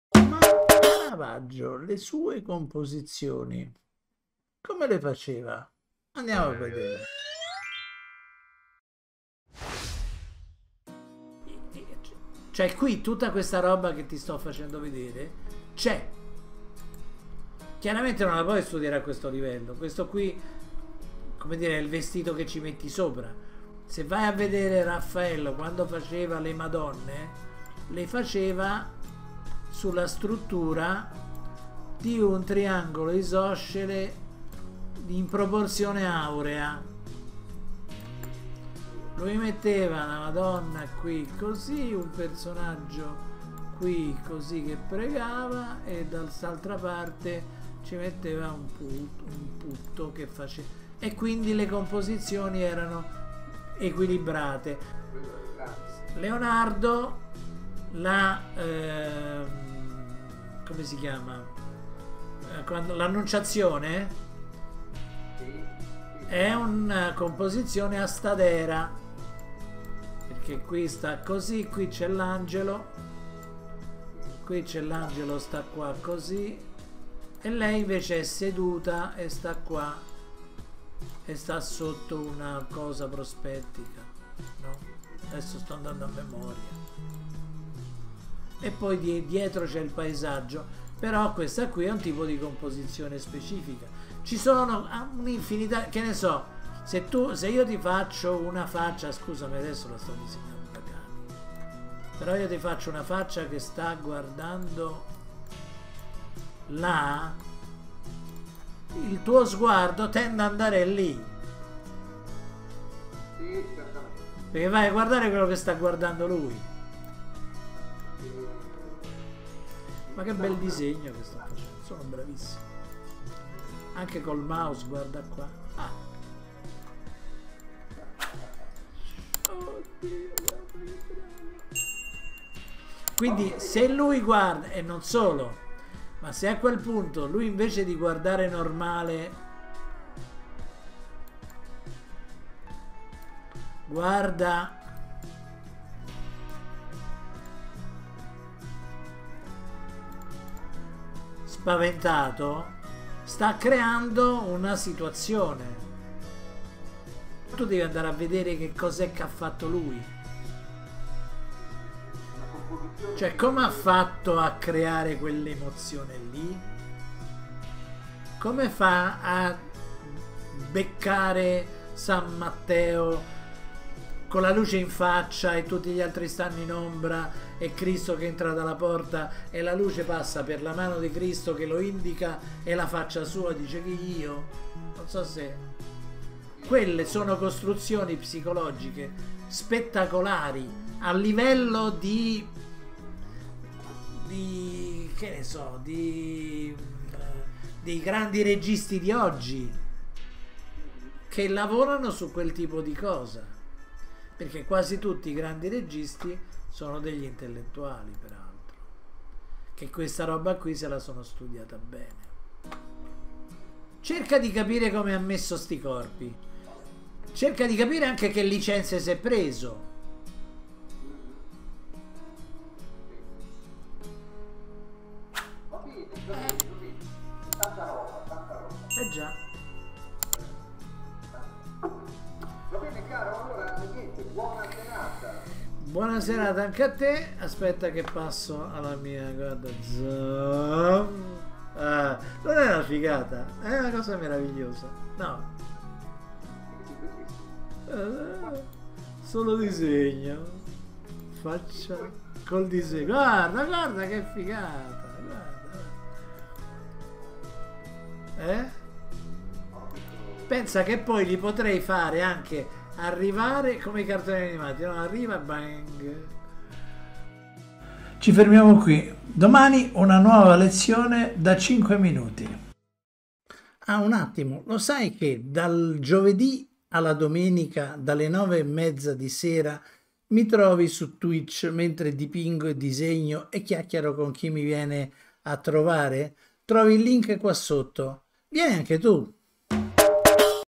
[2] Sveliamo qualche SEGRETO dei quadri di CARAVAGGIO
Le opere dei grandi pittori sono come palcoscenici in cui si svolge una rappresentazione di teatro congelata nel momento più significativo: luci, pose dei personaggi, rapporti tra di loro, tutto è studiato per ottenere il massimo significato e la massima emozione.

* Fai gli esercizi e (se vuoi) ENTRA NEL FORUM per una correzione

composizione in pittura#studio linee implicite#arte#